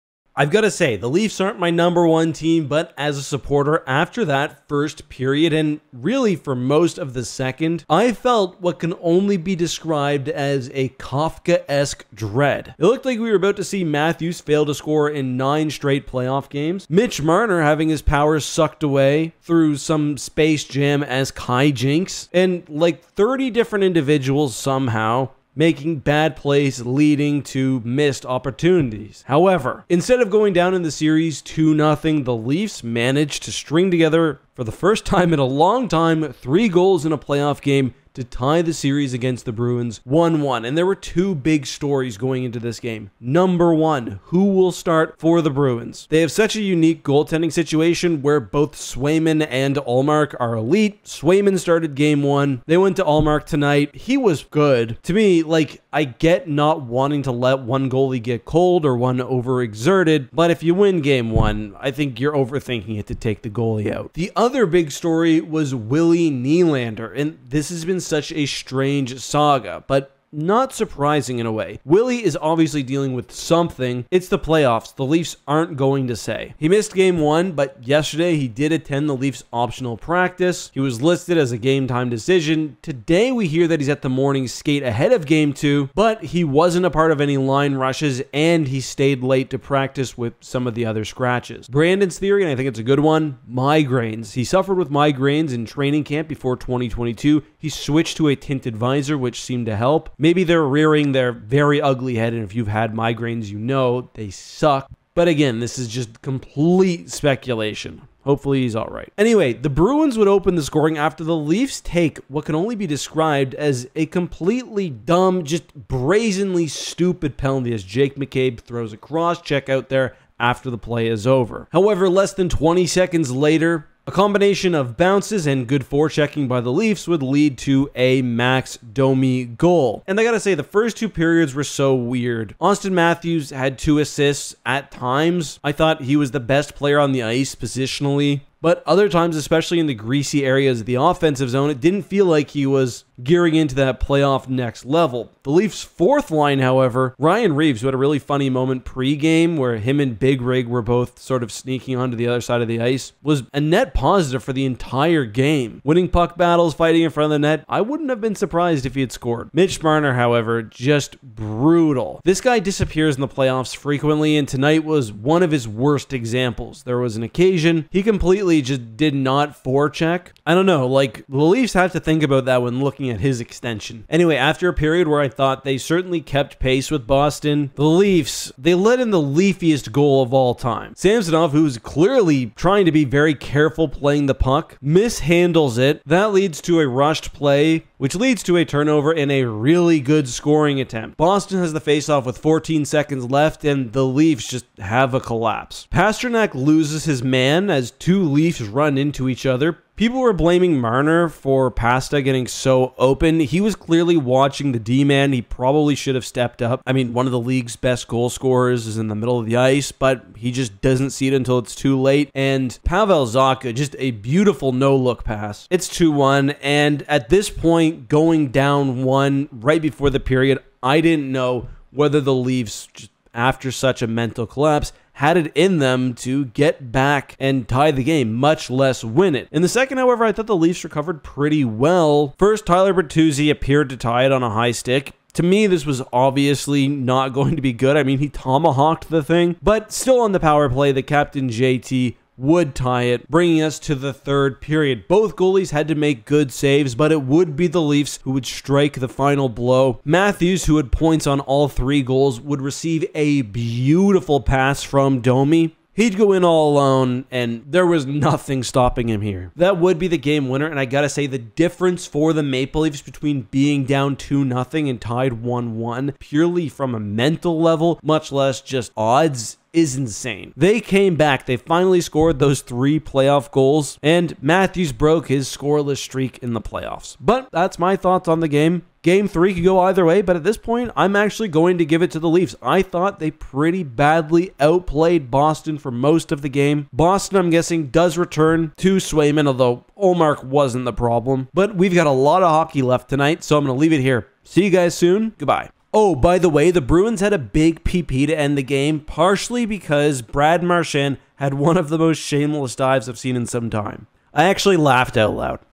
The Leafs needed this BADLY
The Leafs finally score 3 goals in a playoff game as they take game two over the Boston Bruins and now head back to Toronto for games 3 & 4. That and more in today's NHL Hockey Video!